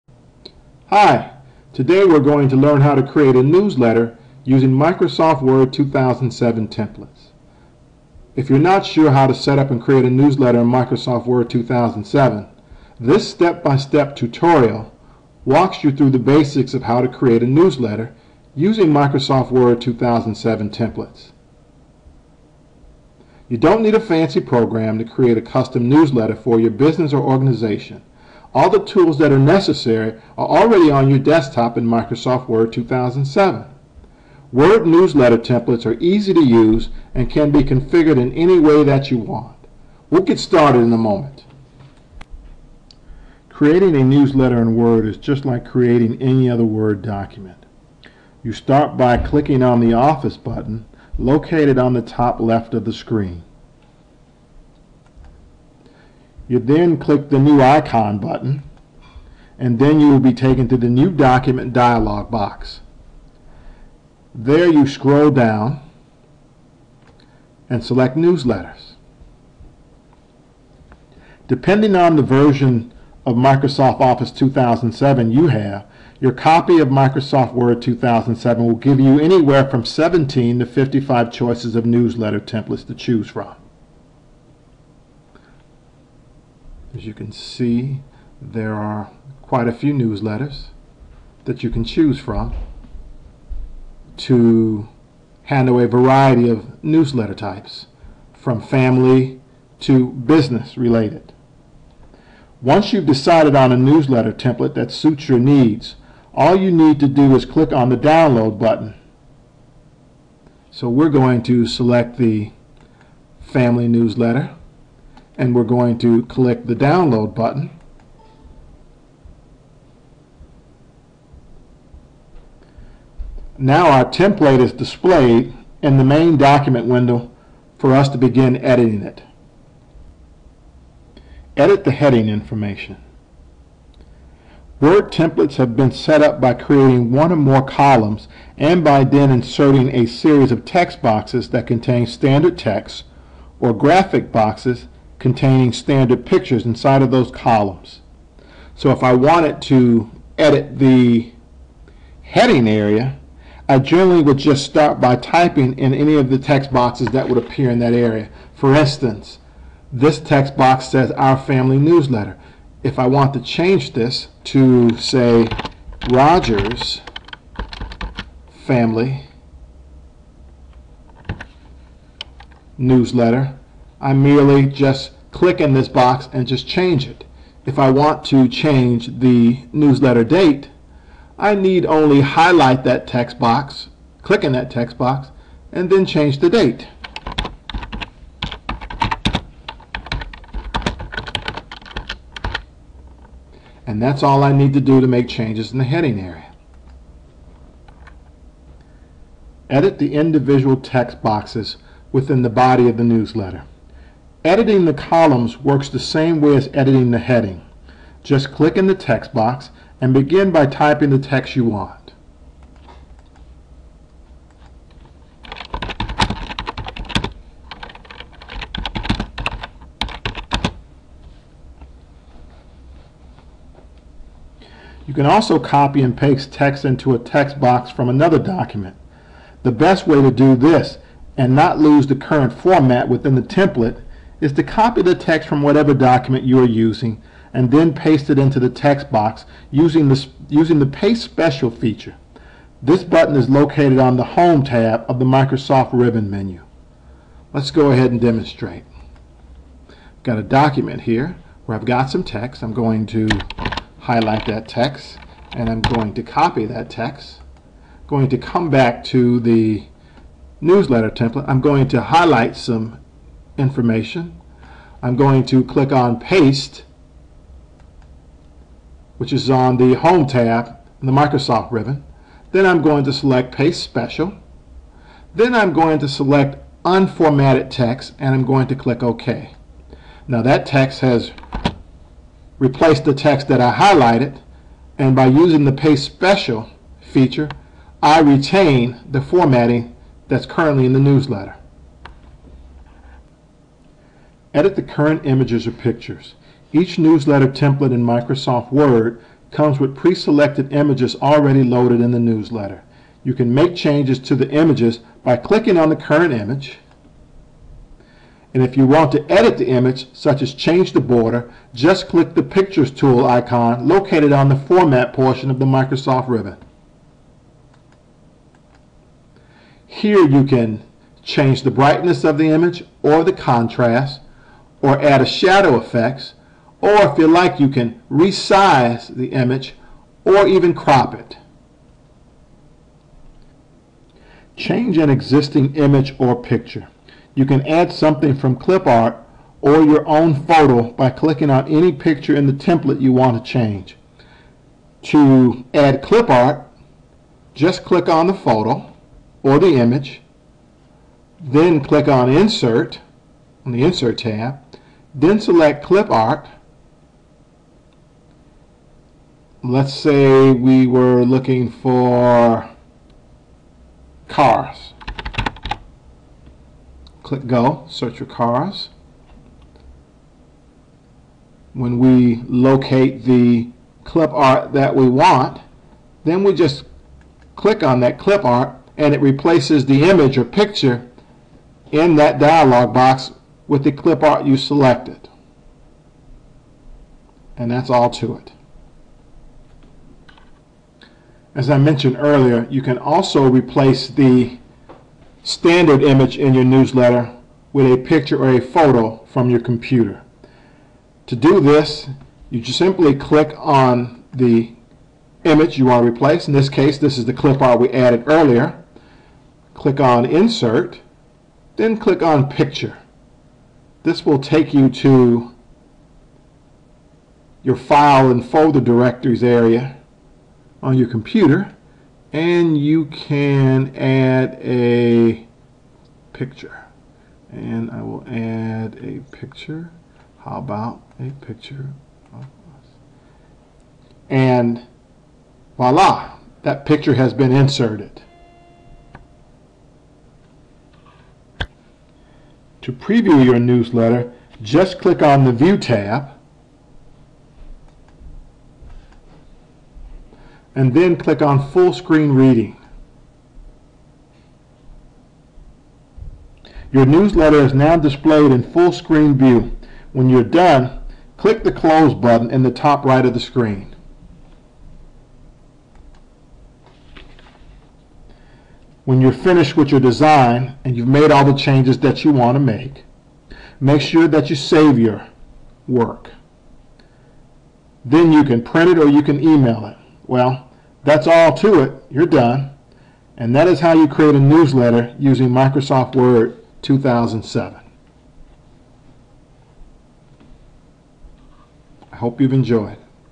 Create a Newsletter With Microsoft Word 2007
Not sure how to set up and create a newsletter in Microsoft Word 2007?  -- This step-by-step tutorial walks you through the basics of how to create a newsletter using the Microsoft Word 2007 templates that are available to you.